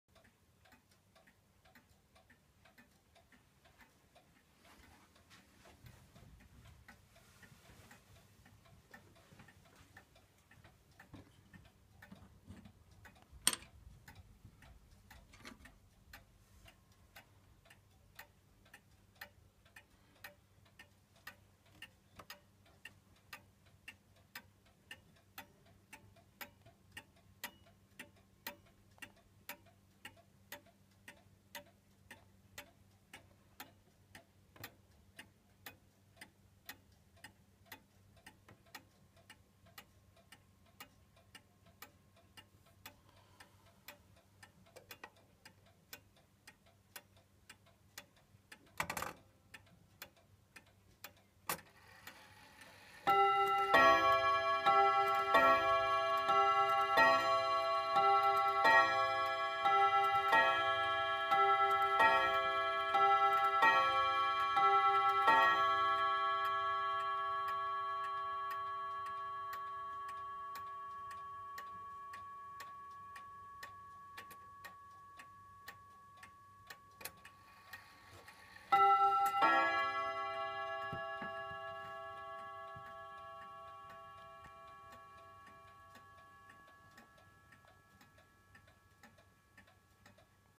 FMS Mauthe wall clock Regulator Wanduhr
Wall clock FMS / Mauthe (Friedrich Mauthe, Schwenningen). 1/2 hour Bim-Bam strike on 3 chimerods, 8 Day Movement. Made around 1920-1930, condition : good

Regulator von FMS Mauthe (Friedrich Mauthe, Schwenningen) mit 1/2 Stunden Bim - Bam Schlag auf 3 Tonstäbe und 8 Tage Werk. Baujahr um 1920 / 1930. Zustand gut.